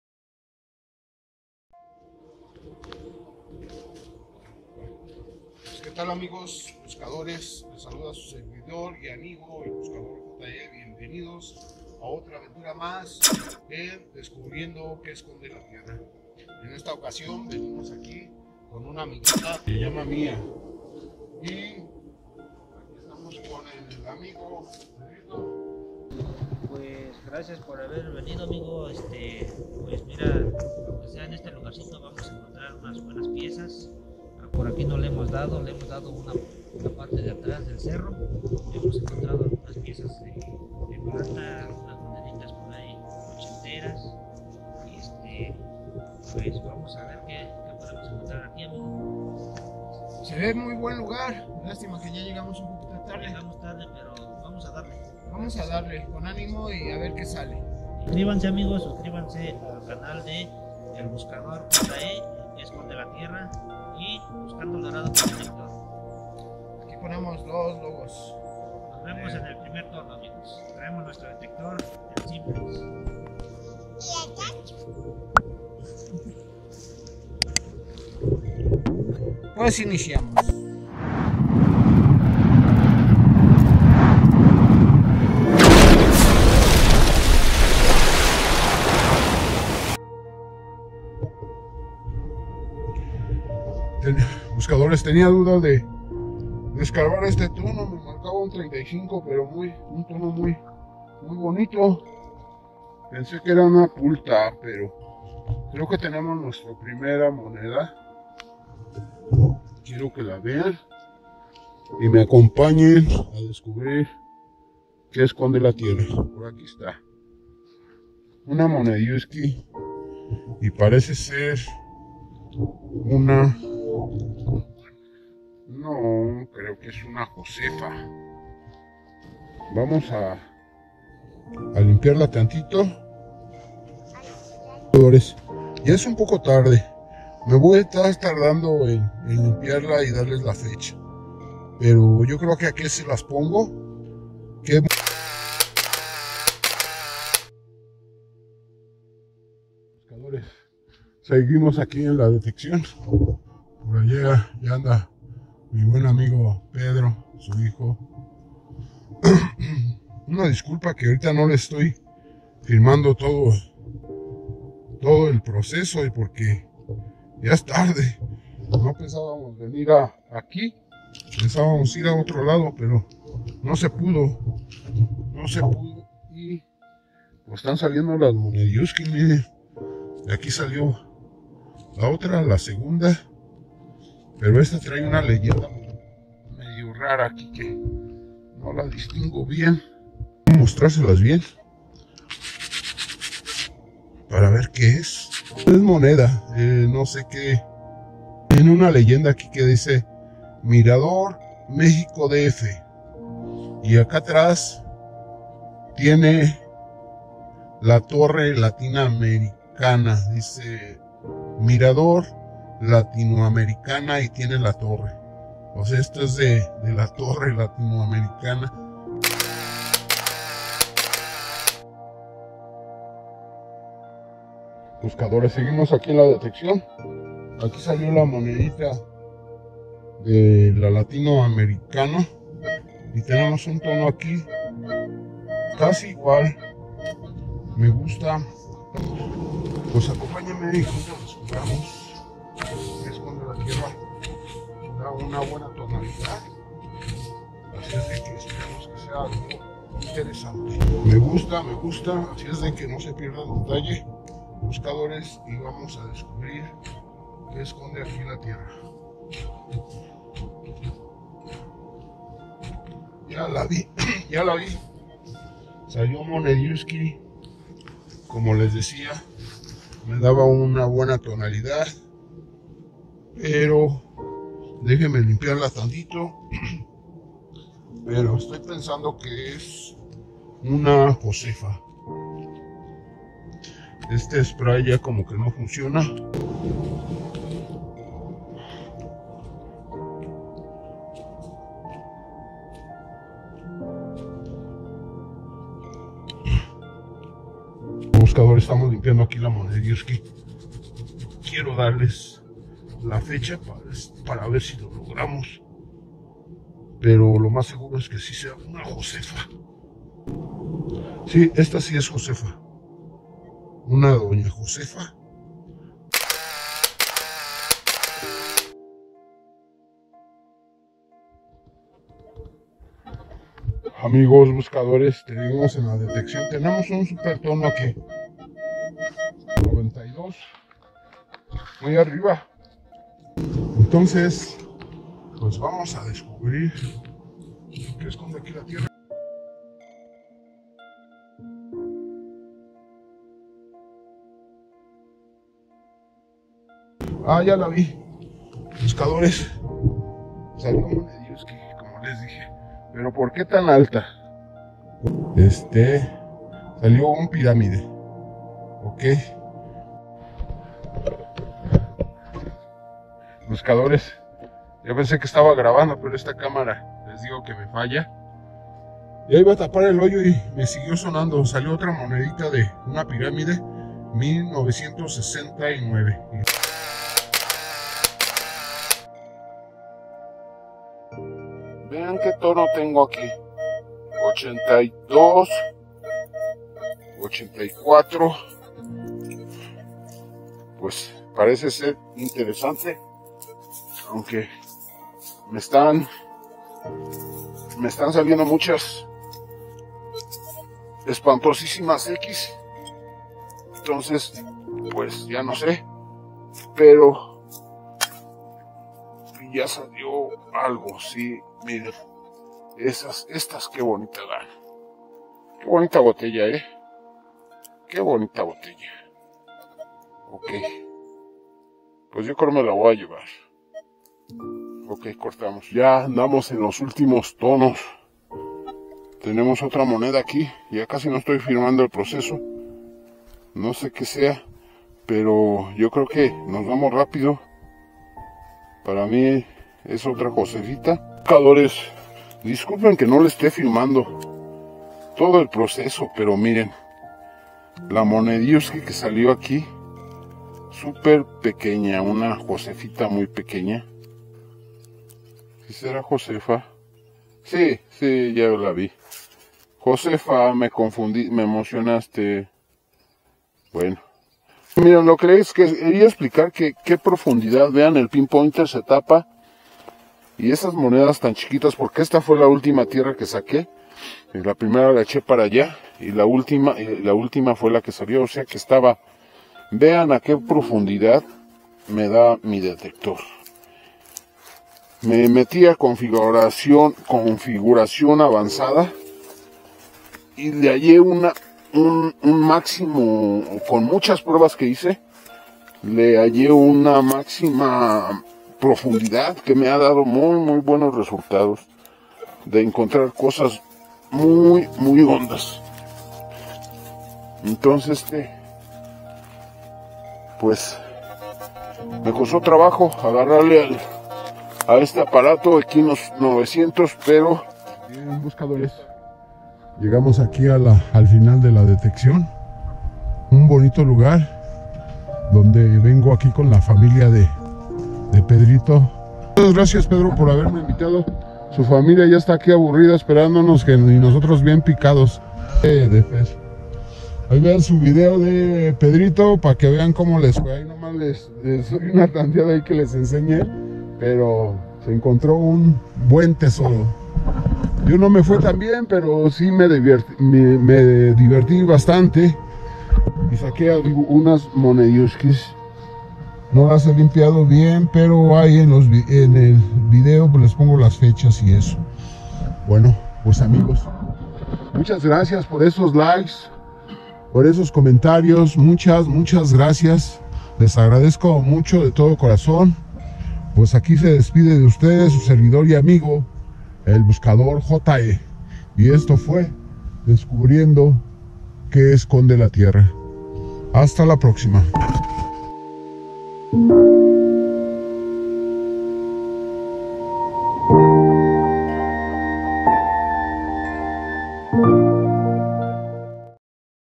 DETECTANDO CON PEDRITO Y SU FAMILIA
YA TARDE PERO LOGRAMOS BUENOS HALLAZGOS EN ESTE BONITO LUGAR CON MUCHA HISTORIA NO SE PIERDAN ESTAS BONITAS AVENTURAS CON SU SERVIDOR Y AMIGO EL BUSCADOR JE EN DESCUBRIENDO QUE ESCONDE LA TIERRA???